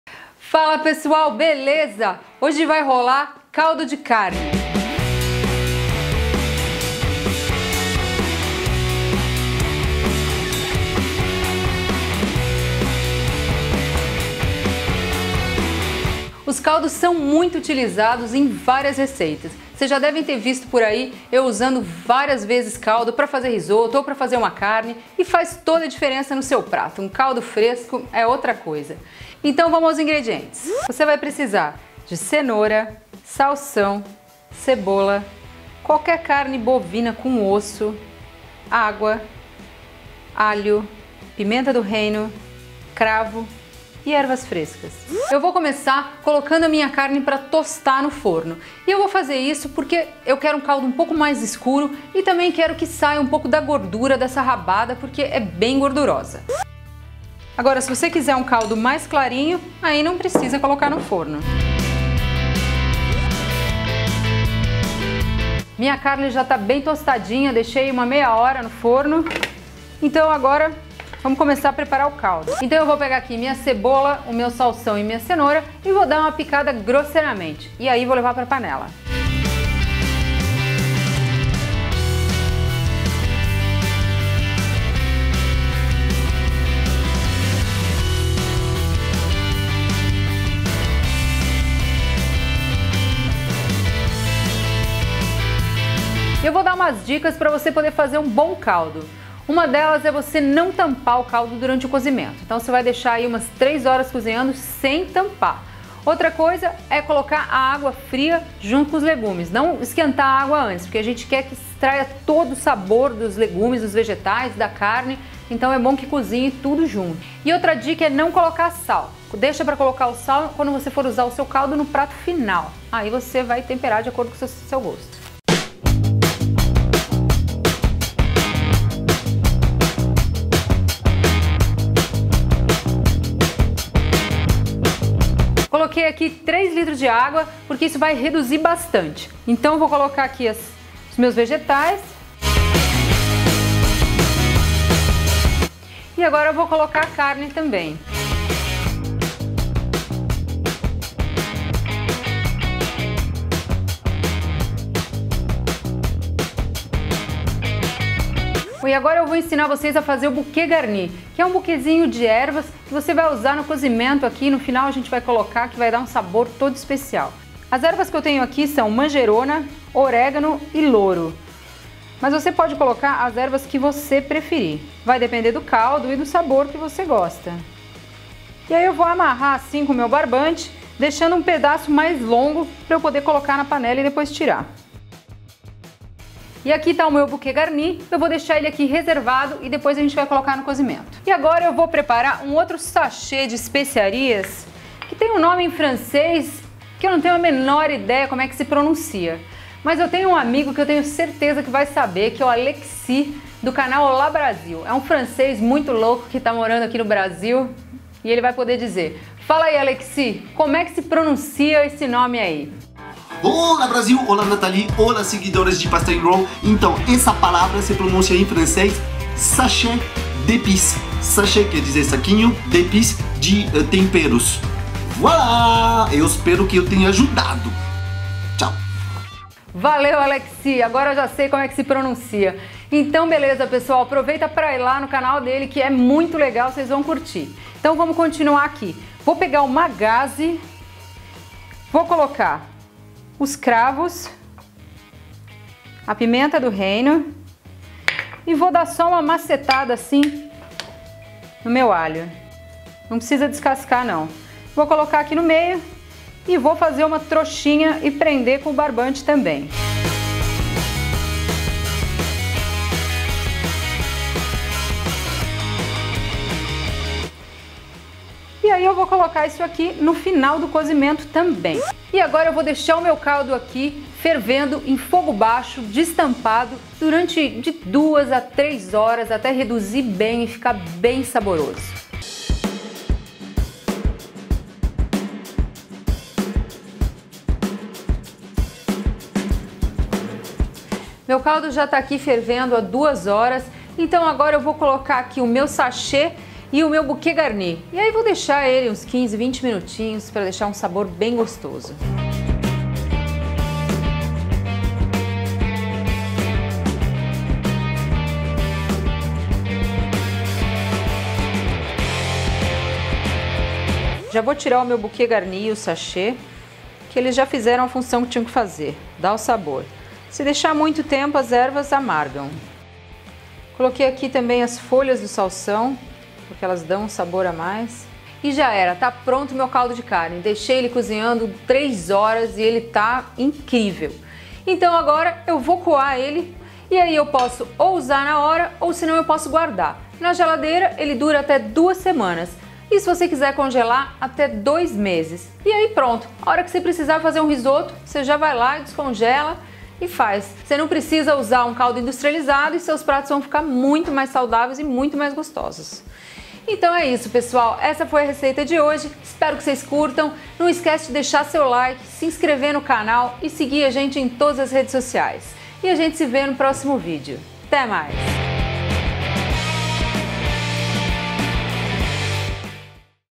CALDO DE CARNE (BRODO) - PASTA AND ROLL
Neste vídeo eu ensino como fazer caldo de carne, uma receita usada no preparo de muitos pratos como risoto, ensopados, sopas etc.